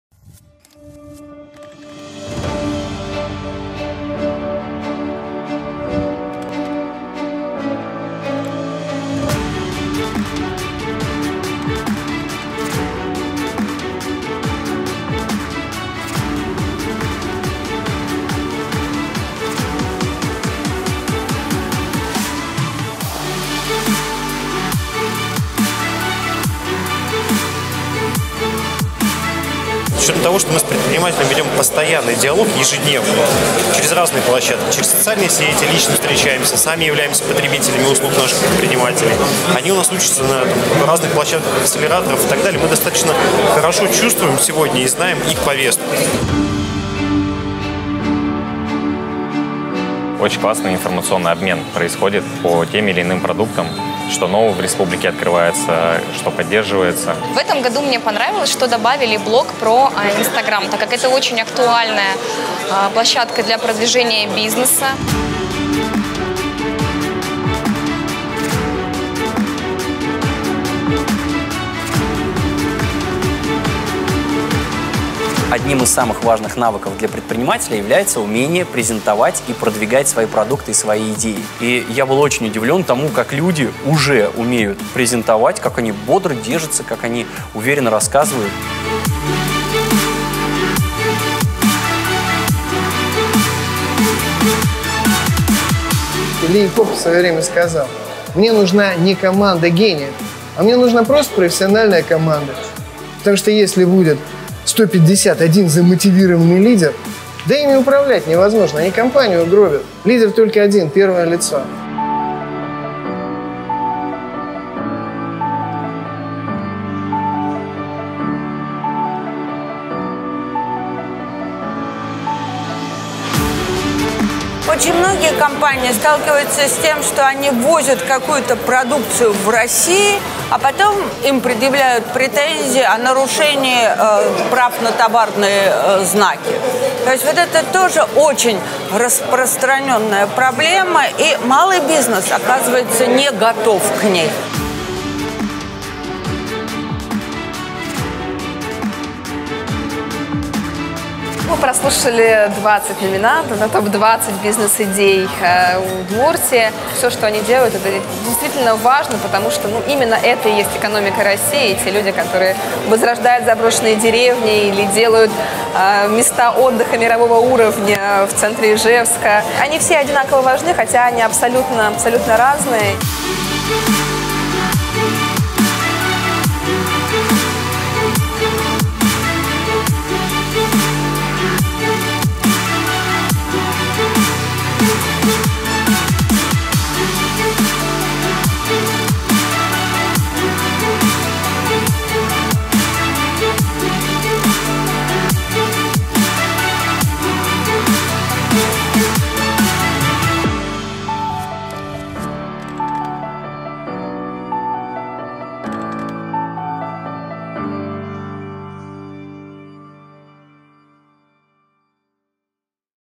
Форум Сделано в Удмуртии 2019
24 и 25 октября в Ижевске прошел третий по счету форум «Сделано в Удмуртии».

Форум 2019 года — это больше 1500 предпринимателей и 29 тематических площадок под различные запросы бизнеса. Делимся итогами мероприятия и отзывами участников.

Мероприятие проводится Министерством экономики УР, Центром «Мой бизнес», в рамках реализации Национального проекта «Малое и среднее предпринимательство и поддержка индивидуальной предпринимательской инициативы». При поддержке Министерства экономического развития РФ.